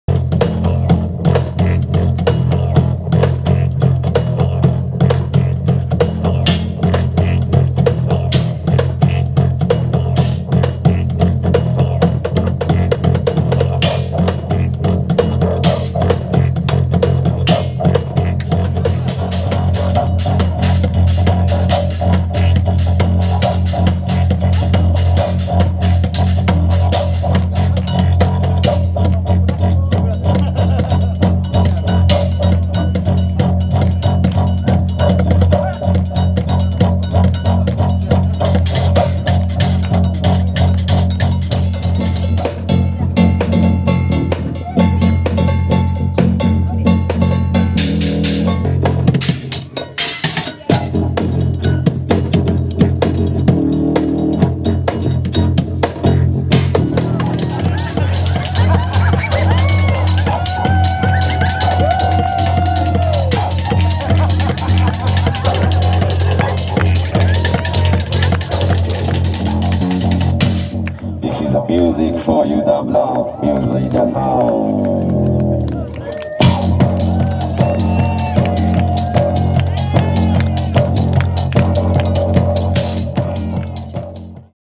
廃材サウンドが室戸に響く、バケツドラマーＭＡＳＡさん野外ライブ
アボリジニの管楽器「ディジュリドゥ」を吹きながら、廃材で組んだドラムセットをたたく―。バケツドラマーＭＡＳＡさんの野外ライブが、室戸市の炭窯「炭遊」で開かれました。